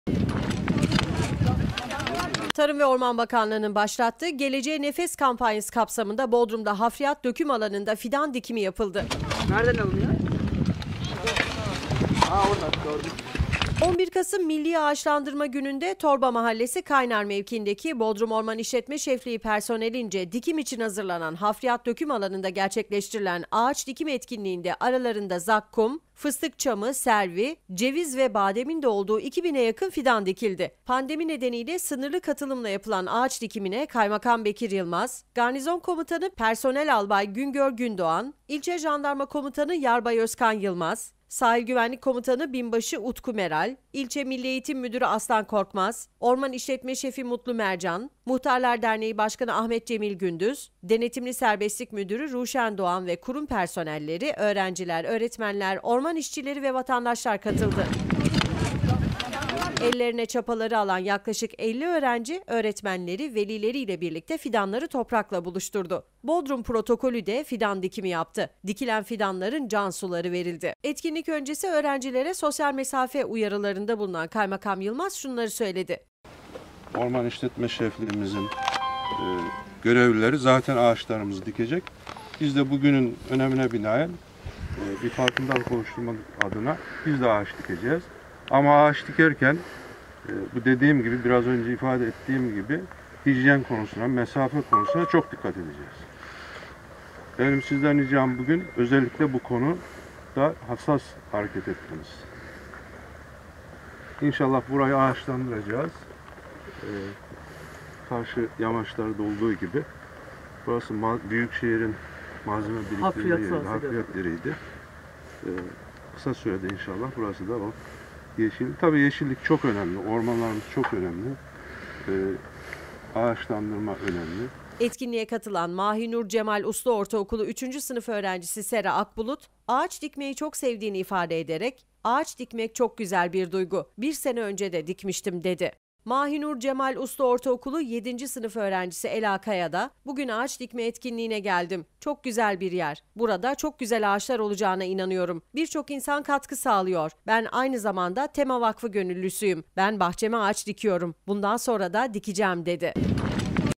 HAFRİYAT DÖKÜM ALANI AĞAÇLANDIRILDI
Hafriyat Döküm Alanı Ağaçlandırıldı...
Tarım ve Orman Bakanlığı'nın başlattığı “Geleceğe Nefes” kampanyası kapsamında, Bodrum’da hafriyat döküm alanında fidan dikimi yapıldı.

11 Kasım Milli Ağaçlandırma Günü’nde Torba Mahallesi Kaynar mevkiindeki Bodrum Orman İşletme Şefliği personelince dikim için hazırlanan hafriyat döküm alanında gerçekleştirilen ağaç dikim etkinliğinde aralarında zakkum, fıstık çamı, servi, ceviz ve bademin de olduğu 2 bine yakın fidan dikildi. Pandemi nedeniyle sınırlı katılımla yapılan ağaç dikimine Kaymakam Bekir Yılmaz, Garnizon Komutanı Personel Albay Güngör Gündoğan, İlçe Jandarma Komutanı Yarbay Özkan Yılmaz, Sahil Güvenlik Komutanı Binbaşı Utku Meral, İlçe Milli Eğitim Müdürü Aslan Korkmaz, Orman İşletme Şefi Mutlu Mercan, Muhtarlar Derneği Başkanı Ahmet Cemil Gündüz, Denetimli Serbestlik Müdürü Ruşen Doğan ve kurum personelleri, öğrenciler, öğretmenler, orman işçileri ve vatandaşlar katıldı.

Ellerine çapaları alan yaklaşık 50 öğrenci, öğretmenleri, velileri ile birlikte fidanları toprakla buluşturdu. Bodrum protokolü de fidan dikimi yaptı. Dikilen fidanların can suları verildi.

Etkinlik öncesi öğrencilere sosyal mesafe uyarılarında bulunan Kaymakam Yılmaz, “Orman İşletme Şefliğimizin görevlileri zaten ağaçlarımızı dikecek. Biz de bugünün önemine binaen bir farkındalık oluşturmak amacıyla ağaçlarımızı dikeceğiz. Ağaç dikerken hijyen ve mesafe konusuna çok dikkat edeceğiz. Özellikle bu konuda hassa hareket etmeliyiz. İnşallah burayı ağaçlandıracağız. Yeşillik, ormanlarımız çok önemli. Zaman zaman yangınlar oluyor ama Bodrum’da arazimizin toplamının yüzde 60’ına yakını ormanlık. Bu açıdan şanslıyız. Hem mevcudu koruyacağız hem de ihtiyaç olan yerleri hep birlikte ağaçlandıracağız.”

Etkinliğe katılan Mahinur Cemal Uslu Ortaokulu 3’üncü sınıf öğrencisi Sera Akbulut, ağaç dikmeyi çok sevdiğini ifade ederek “Ağaç dikmek çok güzel bir duygu, bir sene önce de dikmiştim” dedi.

Mahinur Cemal Uslu Ortaokulu 7’nci sınıf öğrencisi Ela Kaya da "Bugün ağaç dikme etkinliğine geldim. Çok güzel bir yer. Burada çok güzel ağaçlar olacağına inanıyorum. Birçok insan katkı sağlıyor. Ben aynı zamanda TEMA Vakfı gönüllüsüyüm. Ben bahçeme ağaç dikiyorum, bundan sonra da dikeceğim" dedi.